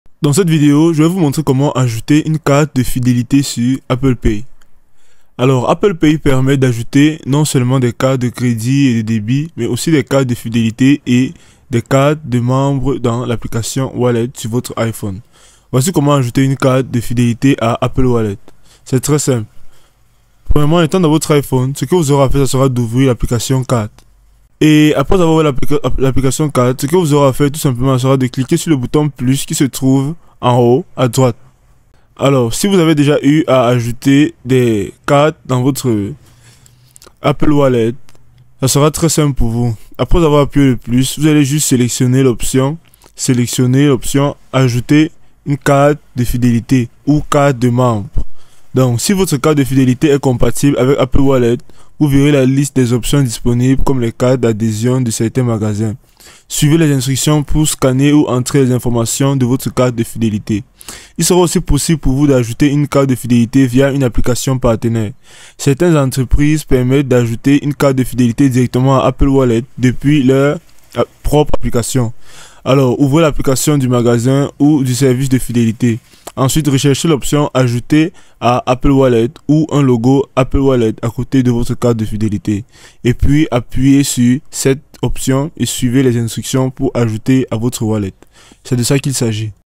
Comment Ajouter Une Carte De Fidélité Sur Apple Pay ( En 2025 )
Je Vais Vous Montrer Comment Ajouter Une Carte De Fidélité Sur Apple Pay